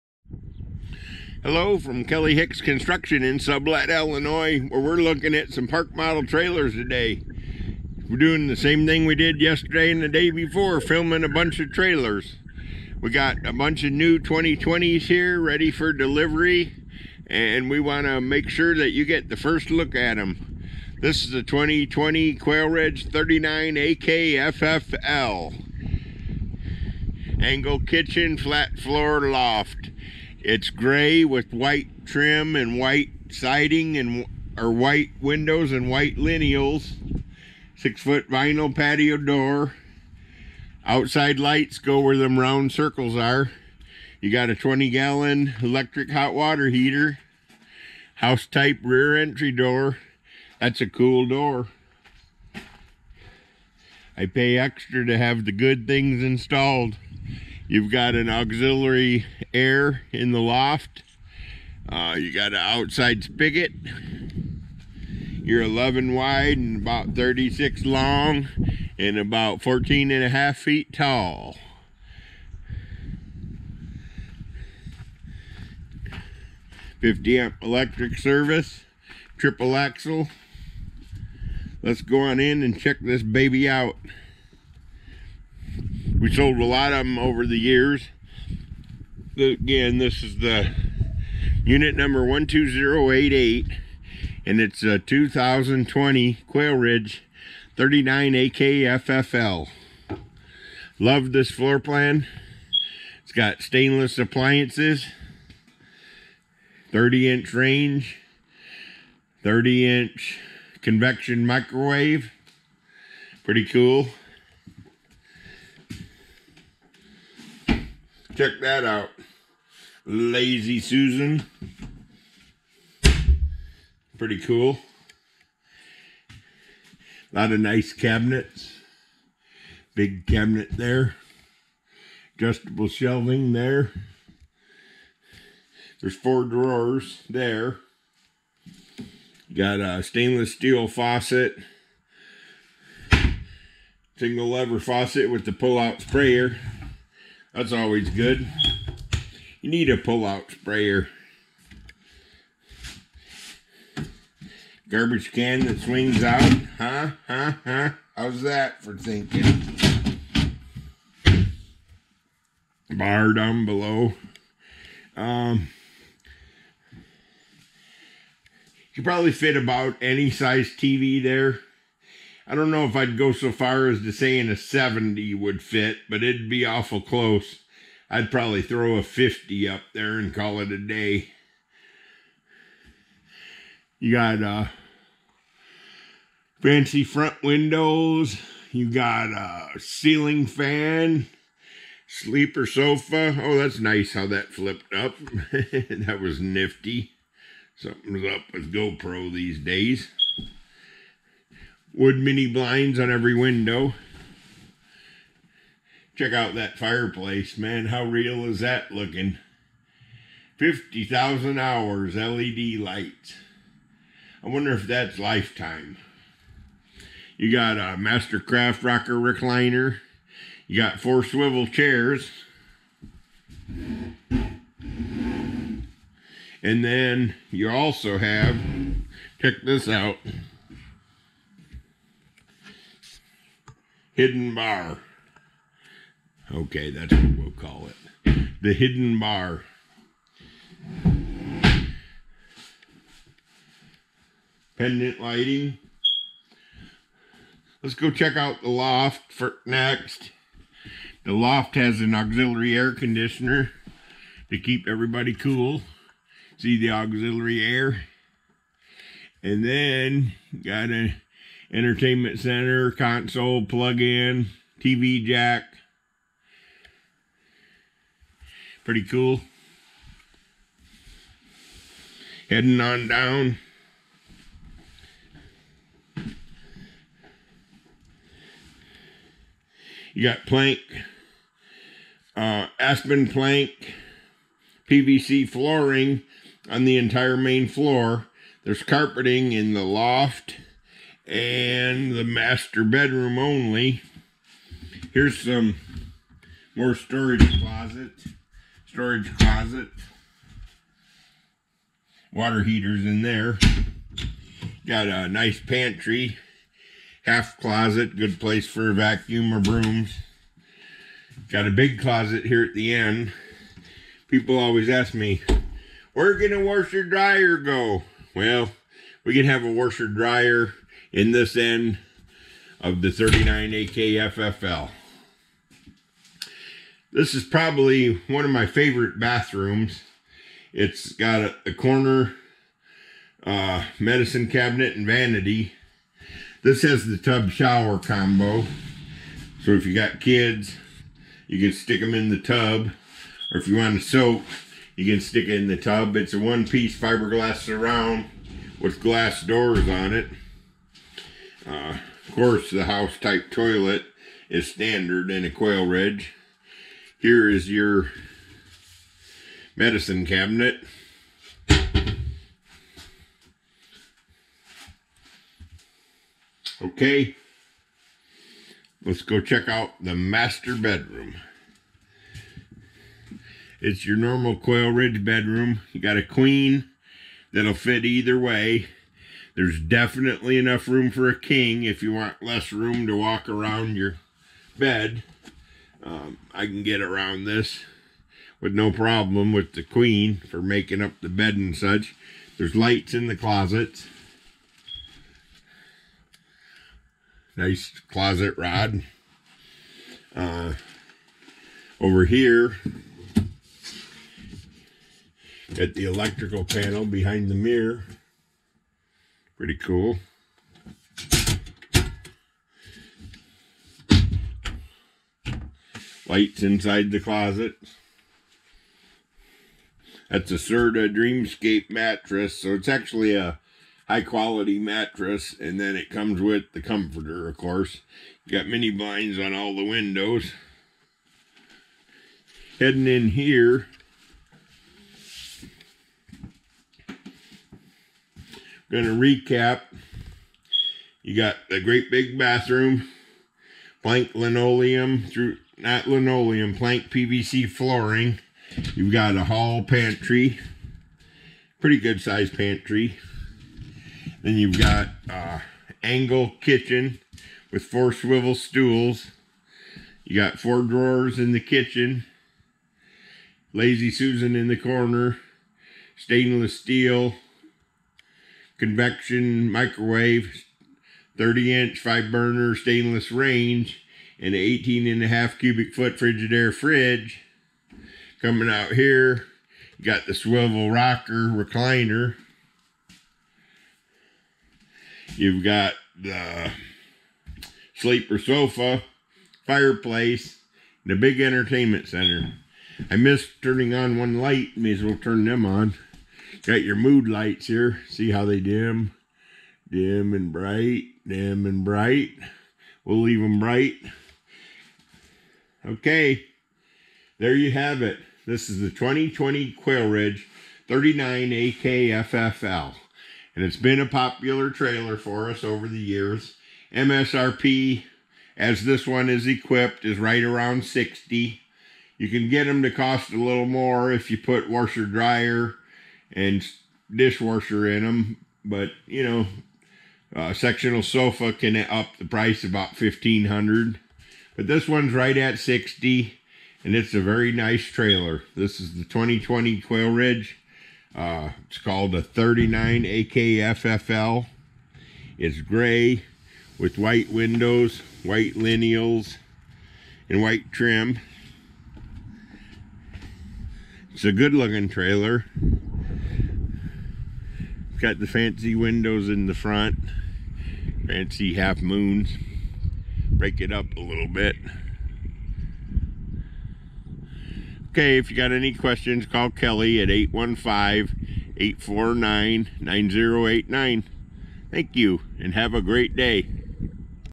Best Tiny House on Wheels: Quailridge 39AKFFL by Forest River or Parkvue 38CKL by Canterbury RV
This Quailridge 39AKFFL Tiny House is by Forest River Park Models.
Canterbury 38CKL Parkvue by DNA Enterprises is a similar Model.
visit @woodhavenlakes for more!